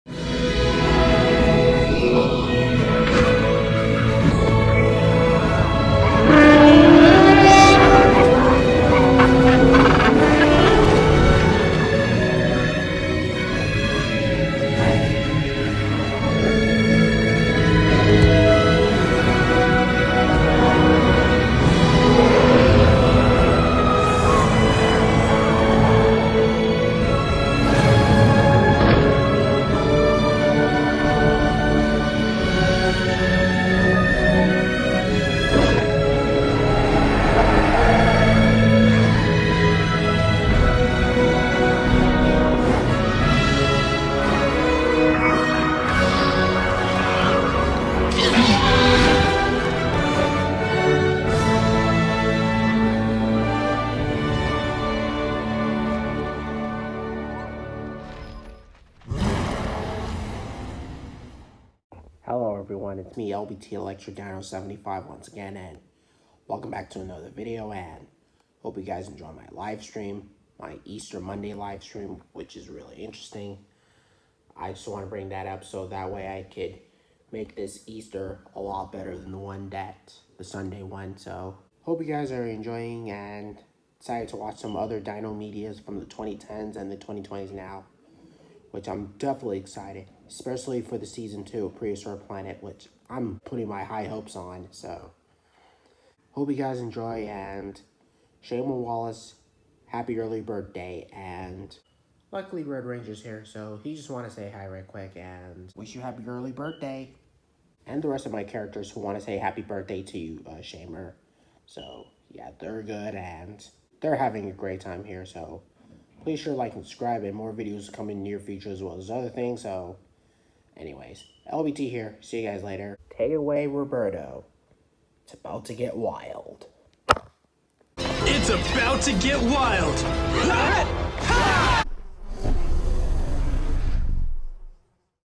LBT Mini Vlog: Just Chilling on this Easter Monday in 2023
I was chilling in my area and watching our dinosaur media and other classic Disney media from my childhood because those were nostalgic and very impressive back then and I’m still excited for Prehistoric Planet Season Two coming out so we could see what they’re going for.